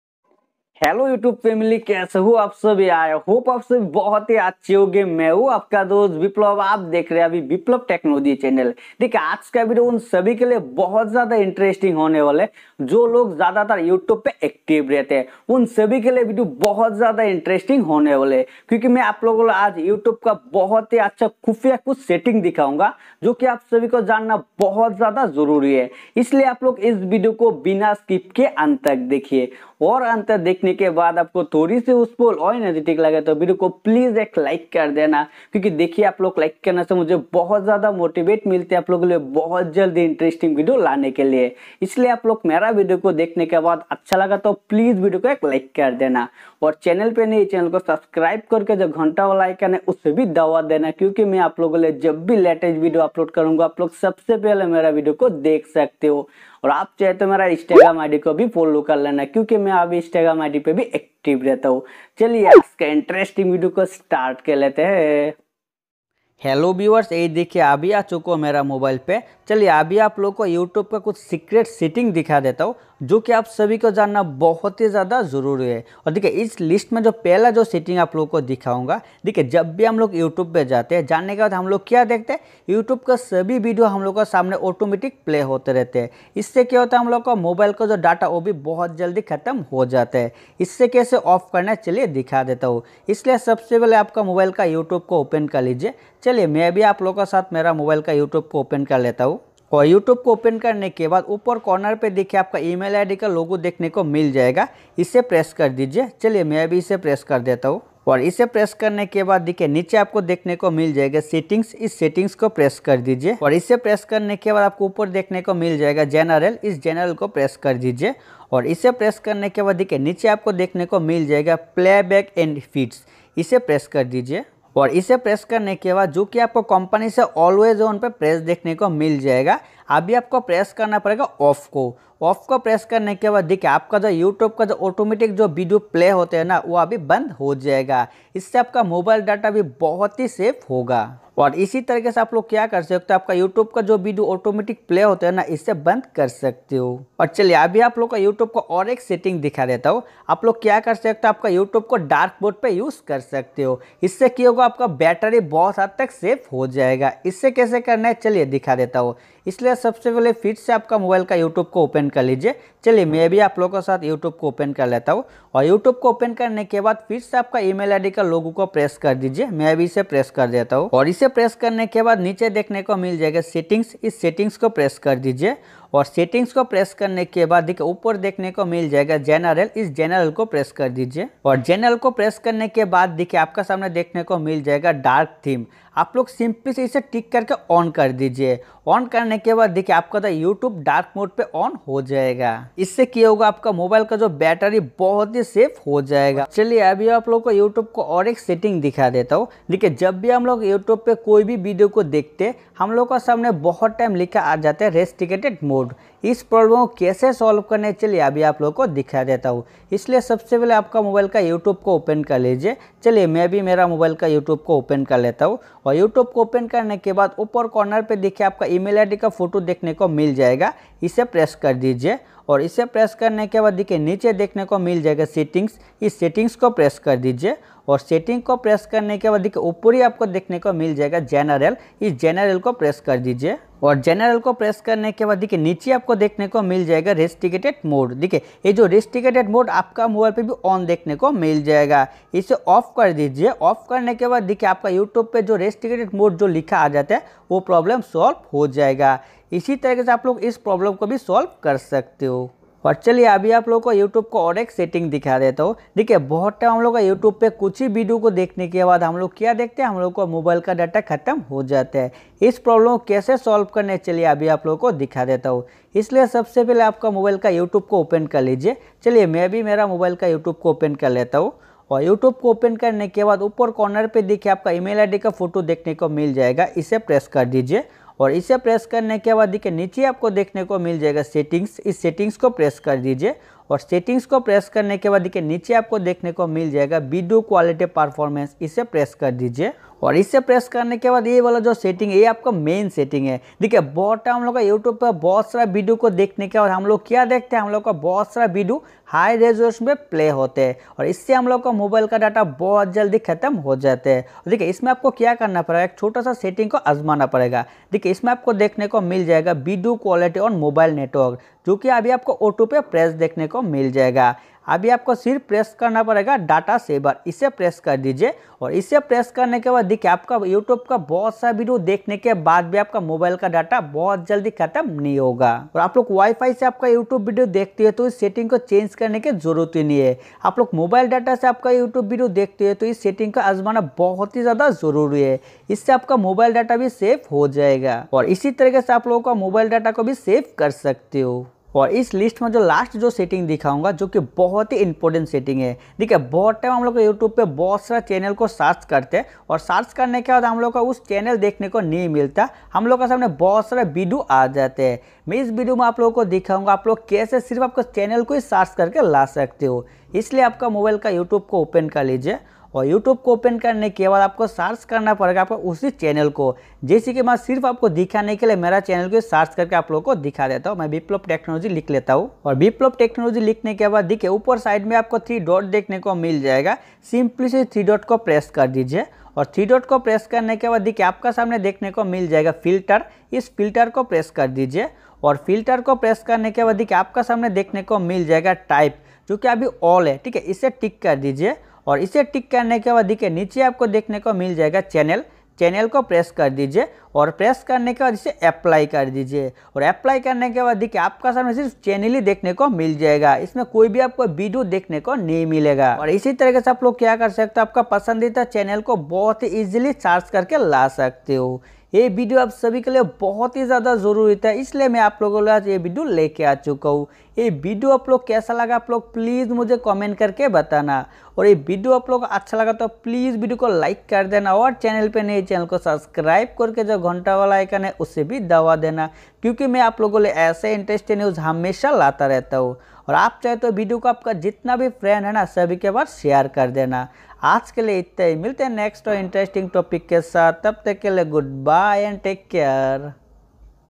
Top 5 Secret Features on YouTube | YouTube Hidden Tips & Tricks | YouTube 5 Dhasu Hidden Tips | BT
YouTube ki Kuchh bahut hi important setting ke bare mein baat karne wali ho is video main

Gear :-👇👇👇👇👇

Tripod :-👉

Mic :-👉

Vlog Gorila Pod :-👉

Ring Light :-👉

Thank You Guys Keep supporting Me🙏